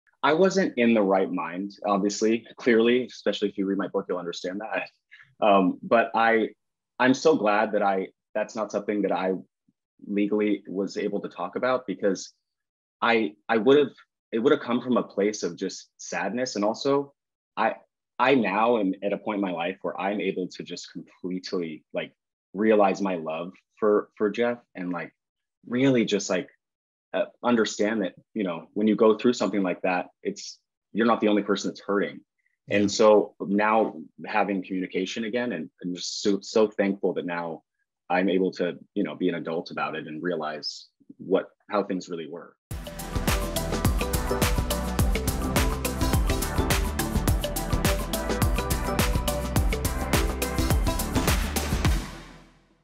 Colton Haynes Shares Feelings on His Divorce | SiriusXM
Hear about how Colton Haynes looks back on his divorce now in an interview with Andy Cohen.